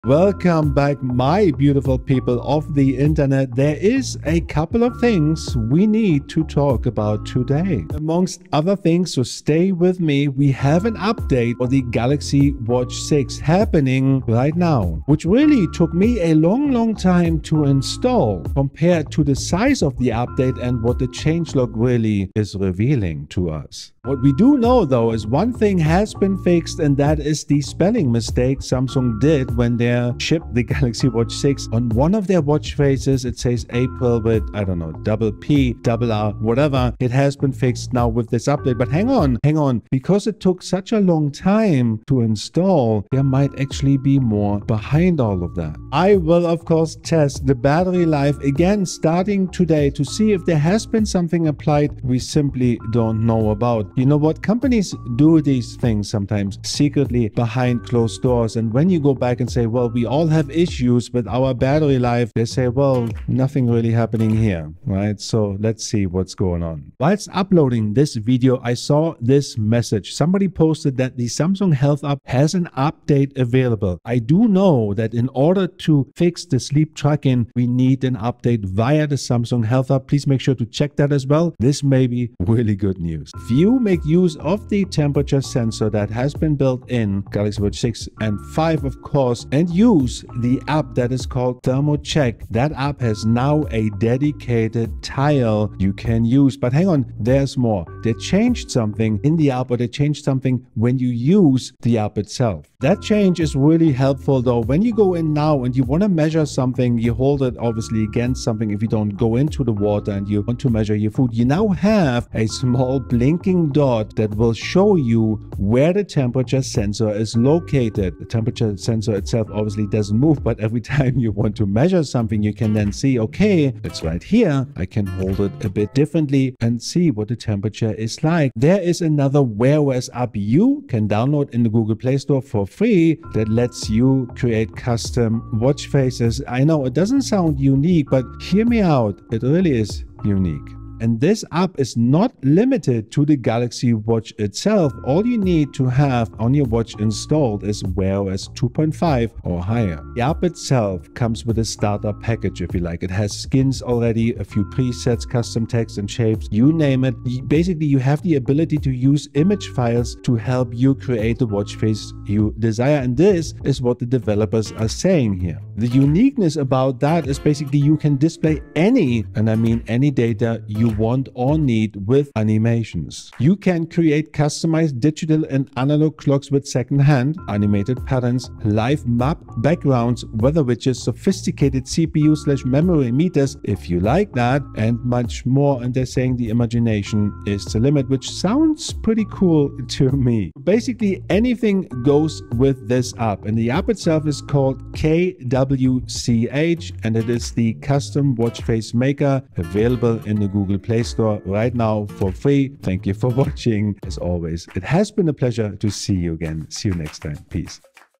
Important: New Fix & Update for Galaxy Watch!
We may finally have a fix for sleep tracking as well as an update & new Apps for the Galaxy Watch 4 & Galaxy Watch 5 & Galaxy Watch 6!